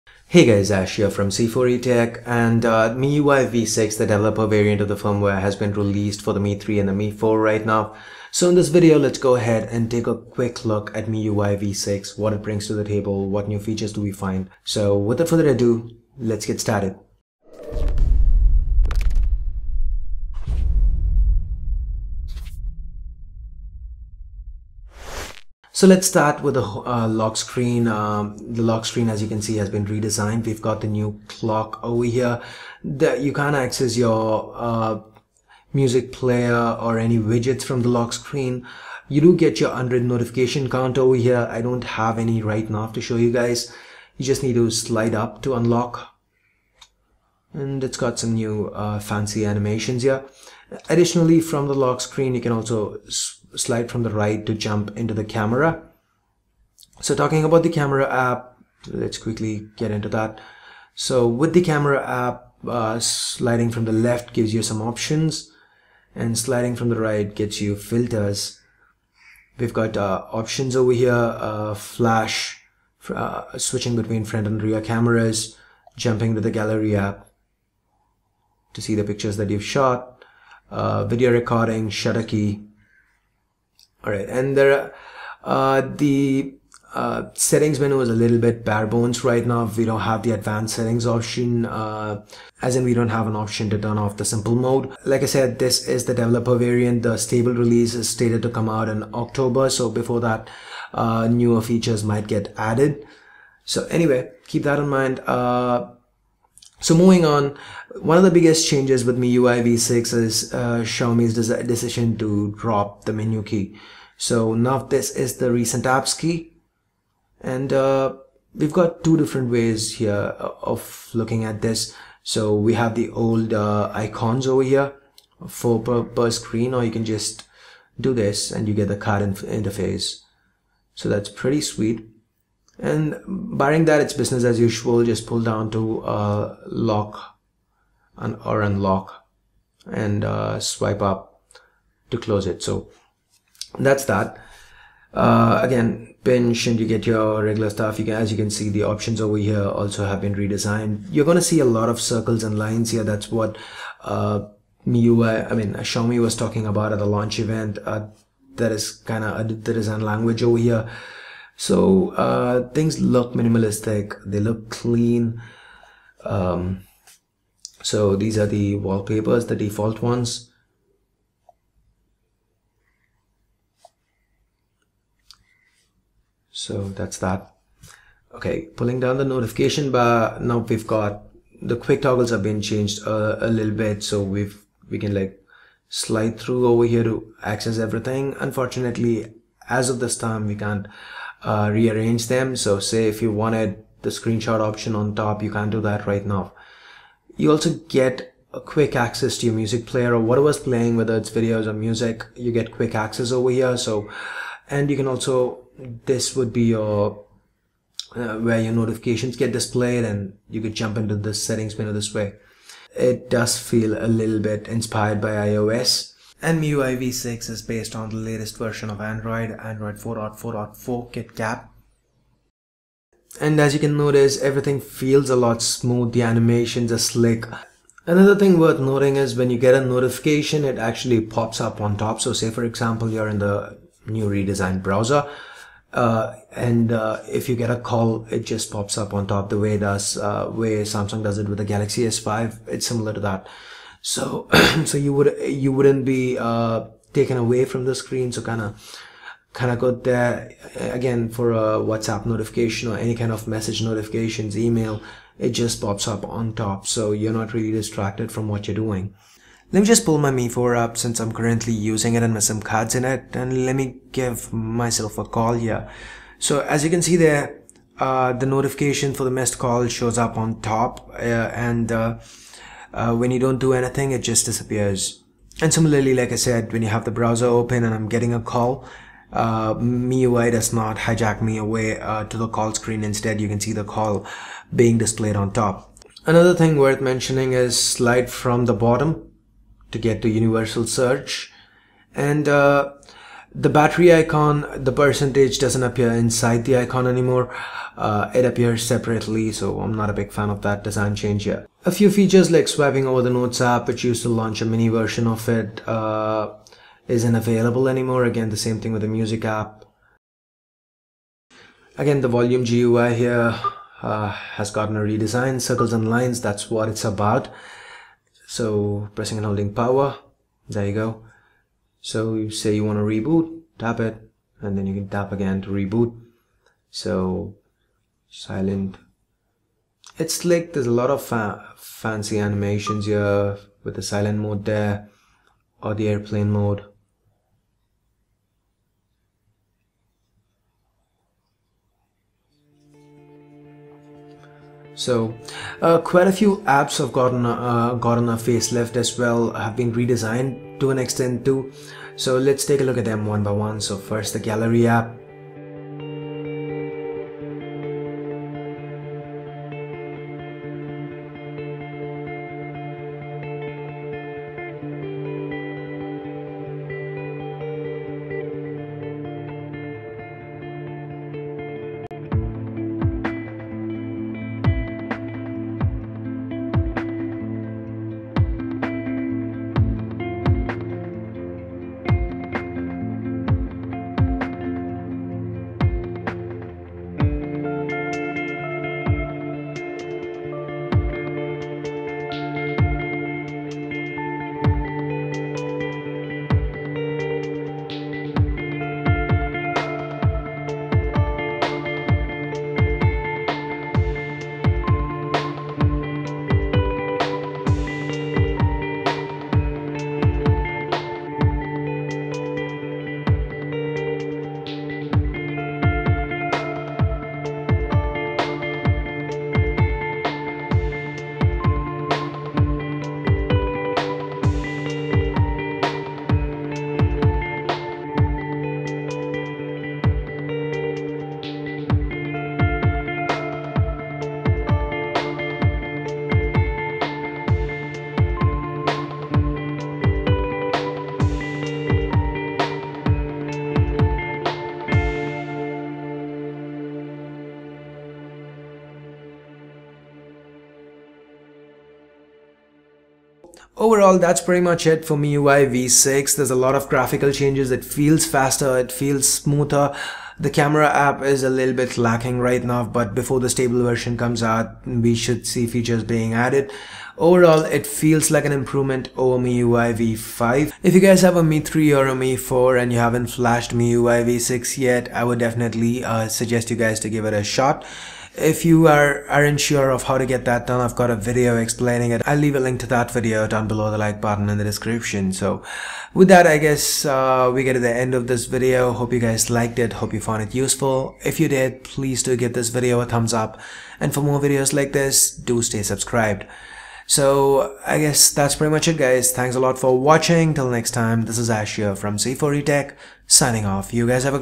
MIUI 6 - What's New? Hands On Review (Xiaomi Mi3 & Mi4)
MIUI v6 brings with it a lot of features and optimizations.

It's based on the latest version of Android, Android 4.4.4 KitKat.

In this video, let's take a look at these features and optimizations that MIUI 6 brings to the table.